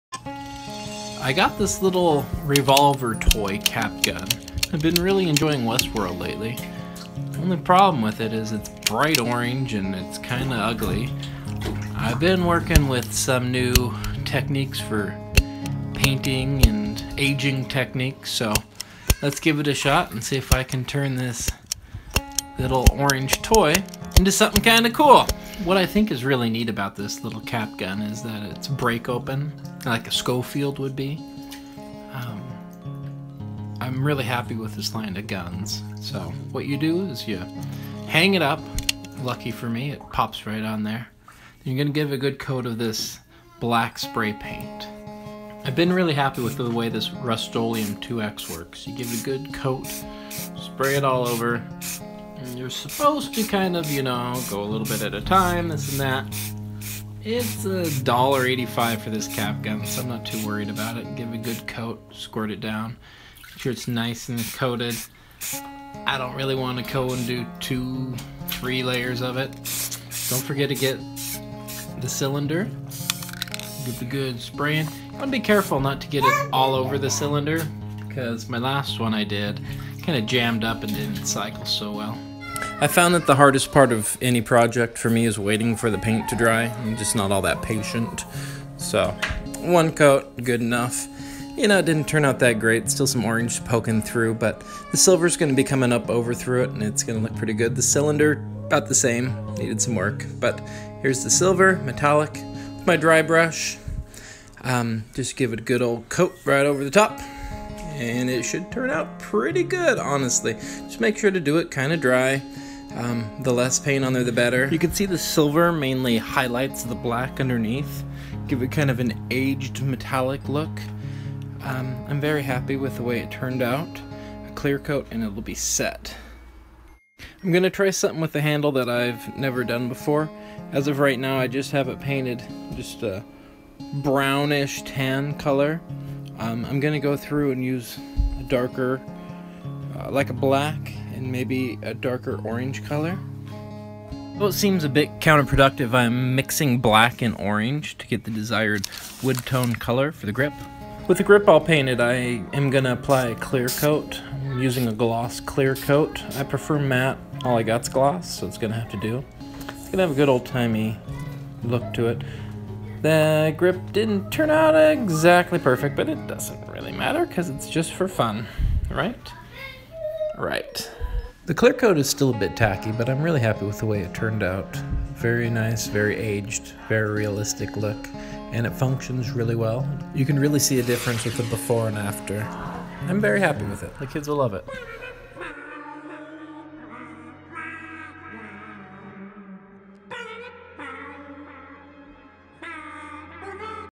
Cap gun makeover.
Not a diy instructional video, just a fun time showing how I painted my pistol. With spray paint, dry brush, and some time I made my revolver pop.